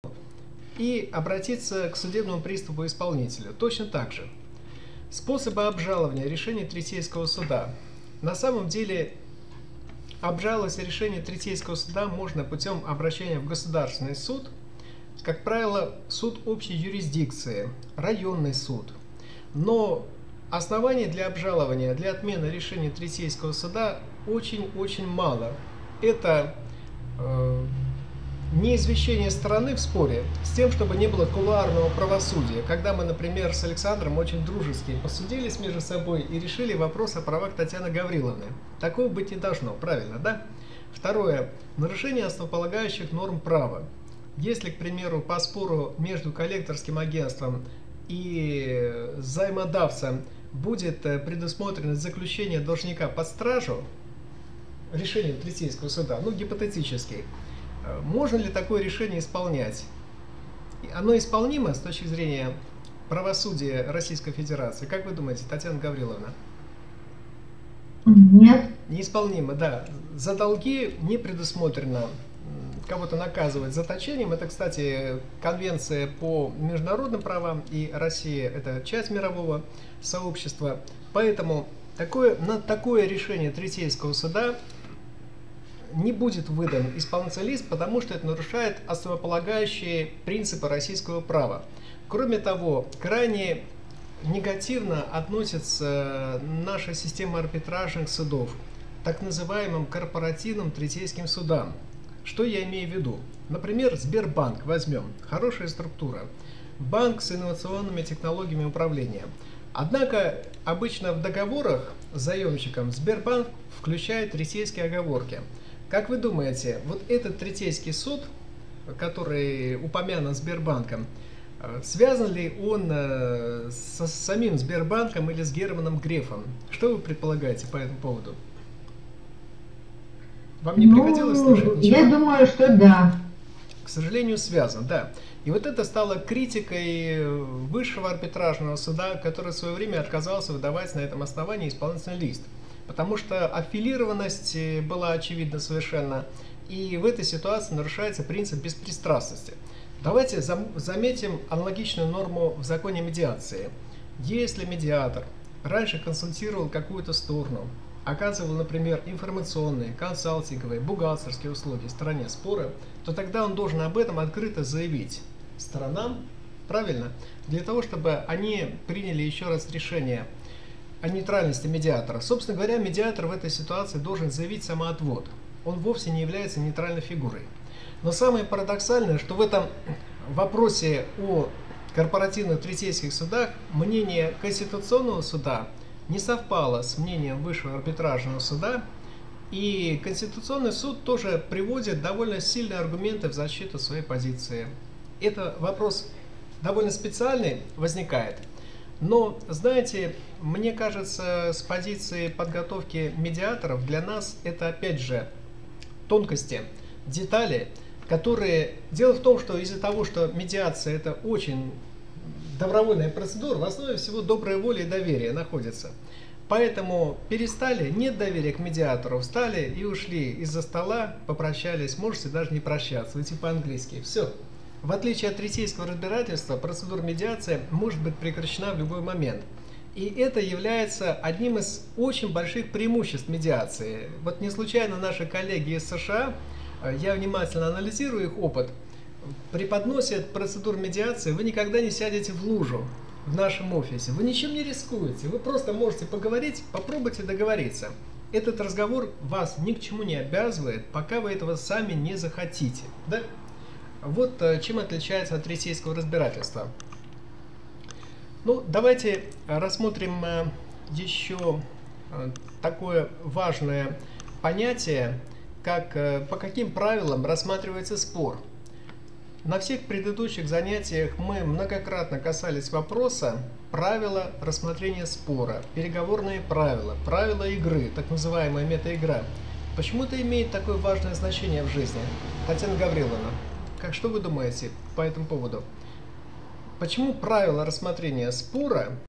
Принудительное исполнение третейского решения
процедуры и медиативные компетенции Кейсы из практики.   Марат Авдыев (С) Сибирский Центр конфликтологии 2015